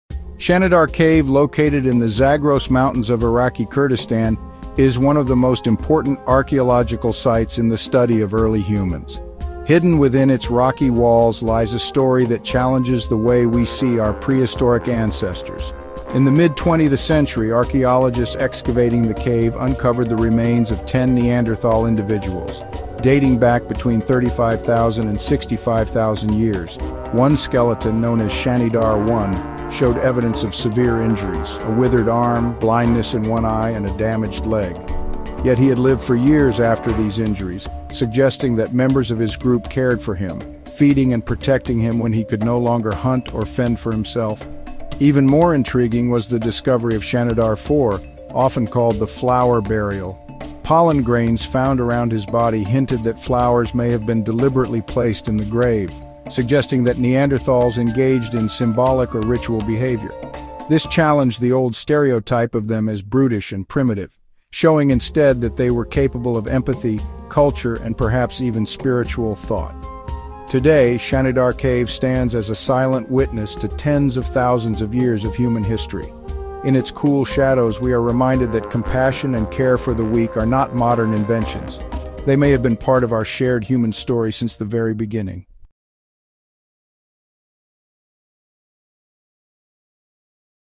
Did Neanderthals Bury Their Dead with Flowers?
Discover the incredible story of Shanidar Cave in Iraqi Kurdistan, a key archaeological site that reshaped our understanding of Neanderthals. Unearthed in the mid-20th century, the remains of ten Neanderthal individuals reveal surprising evidence of compassion and culture. Shanidar 1’s healed injuries suggest group care, while Shanidar 4’s “Flower Burial” hints at ritualistic practices, challenging old stereotypes of Neanderthals as mere brutes. This cave tells a powerful tale: empathy and symbolic thought may have been part of human history tens of thousands of years ago. Dive into the mystery of our ancient relatives and explore how they cared for one another long before modern times.